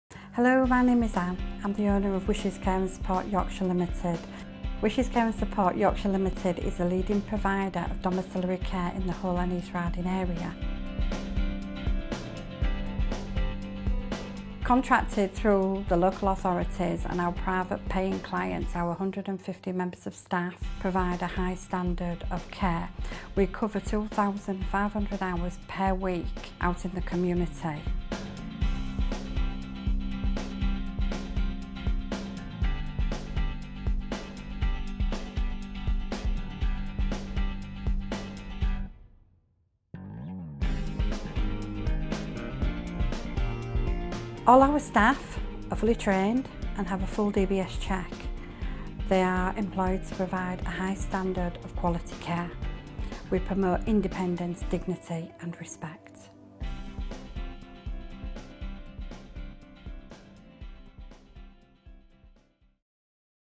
Wishes Care and Support Yorkshire Ltd, Hull and East Riding
At Wishes Care and Support Yorkshire Ltd, we believe in independence, dignity and respect, and our aim is to provide all of our clients the highest level of care in the community.

All of our care workers are fully trained to CQC standard and DBS checked.

We cover the Hull and East Riding area, providing Elderly Care and Complex Care for individuals with physical disorders and health disabilities.